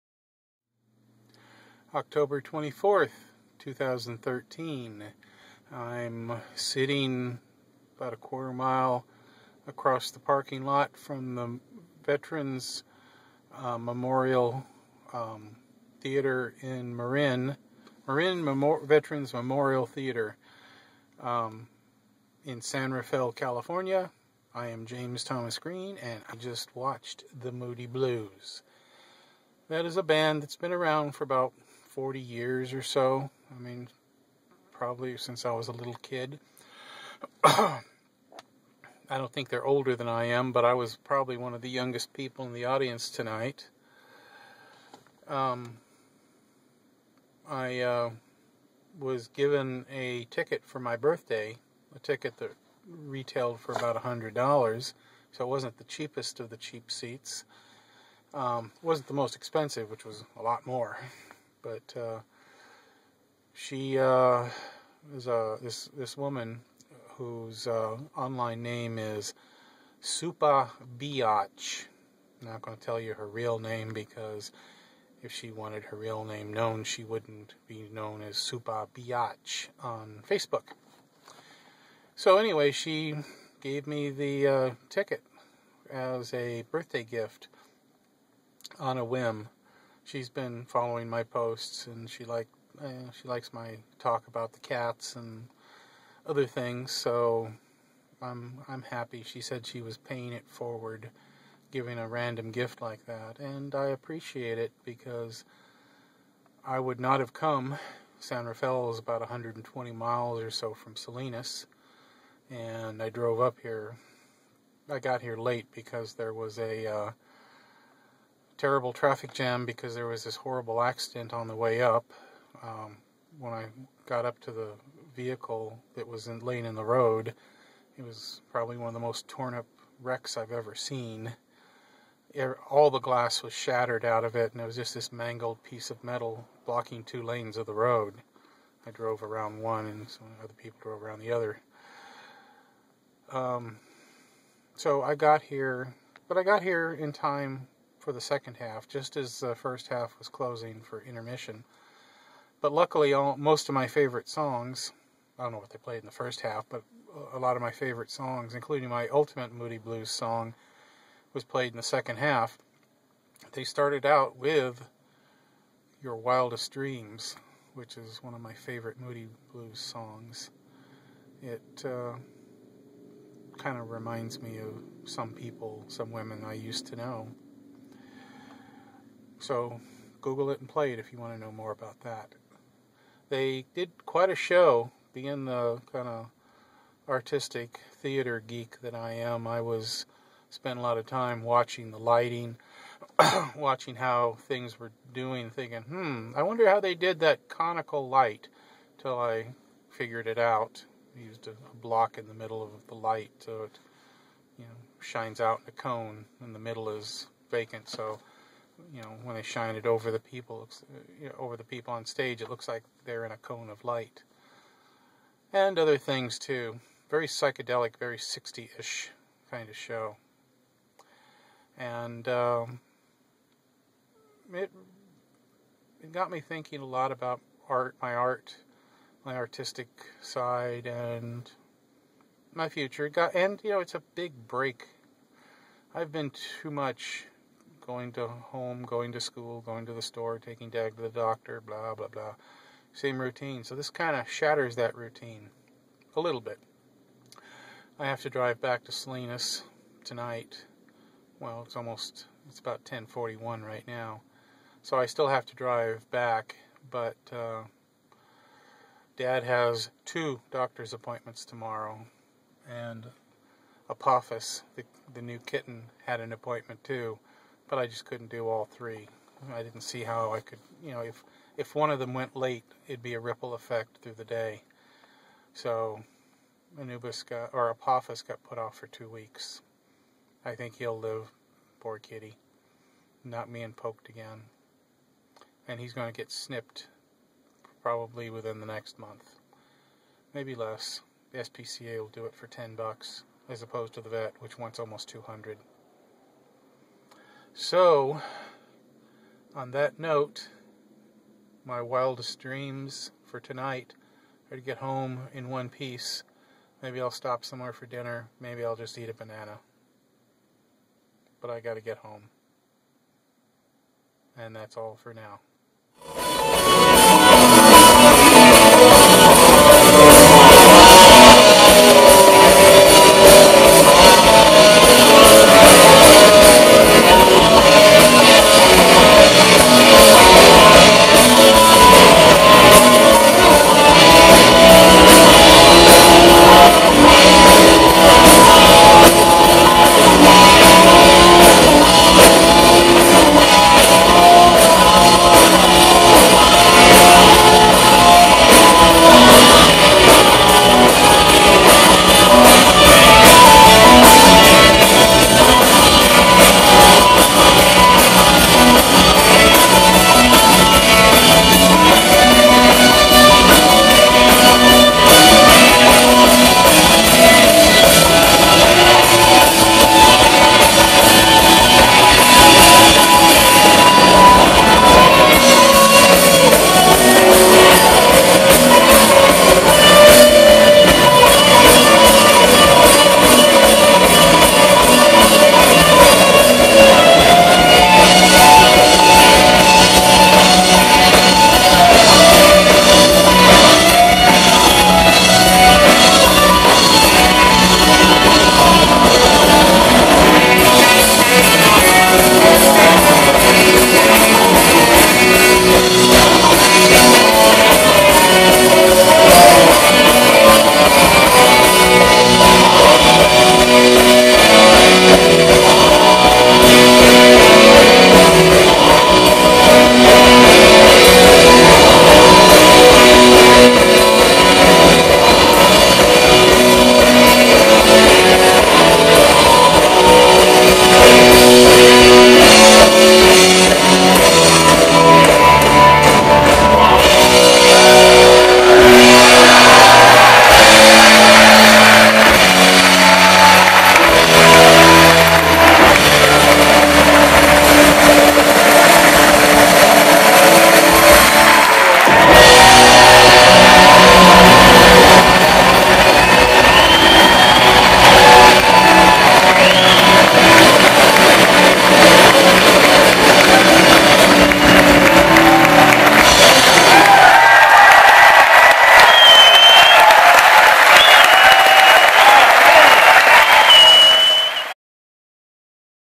JTG Journal | 20131024-My Wildest Dreams
On 24 OCT 2013 I went to see the Moody Blues play at the Marin Veteran's Memorial center. Supah Beeotch gave me the ticket as a birthday present. :D